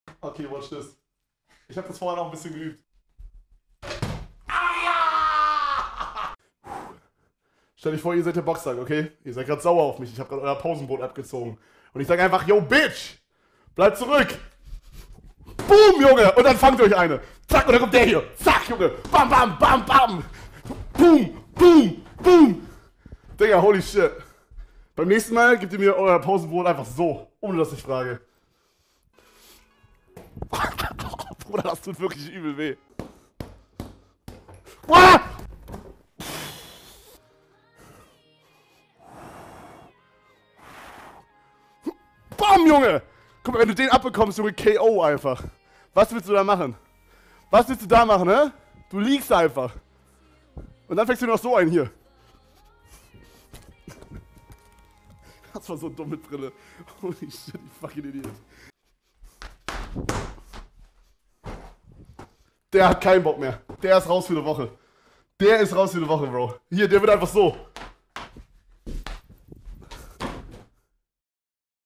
Papaplatte hat jetzt einen Boxsack... - Papaplatte Clips
Kevin zeigt uns seine Skills und Kicks, der neue Boxsack ist sick! / Papaplatte Clips / Sport / G4M

Es werden nur Clips verwendet, die mit hoher Wahrscheinlichkeit nicht in einem zukünftigen YouTube Video von @papaplatte landen werden.